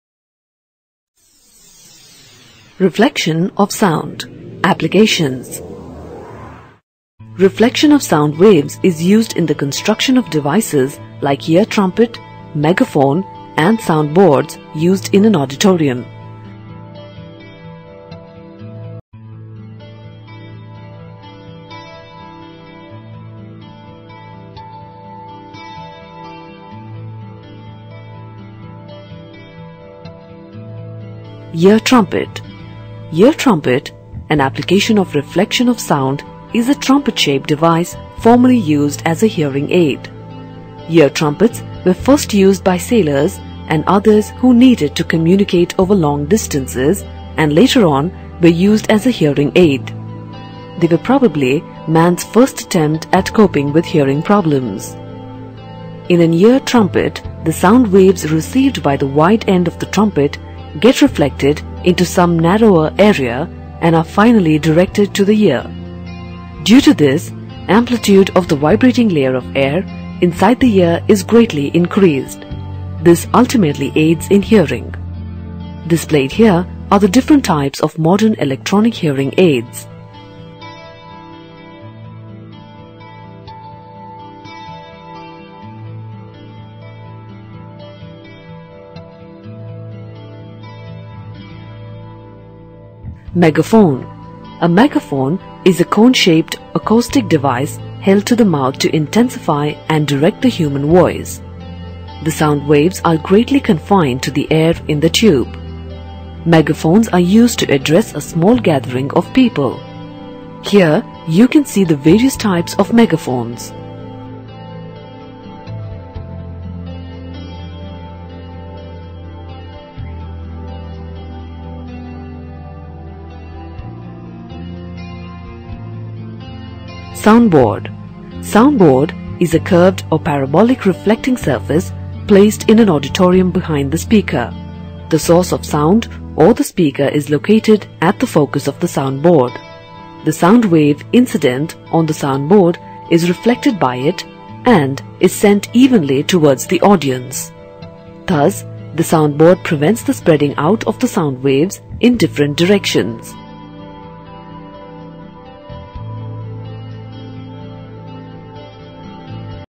Reflection of Sound
Sound Reflection

The reflection of the sound follows the law "angle of incidence equals angle of reflection", sometimes called the law of reflection.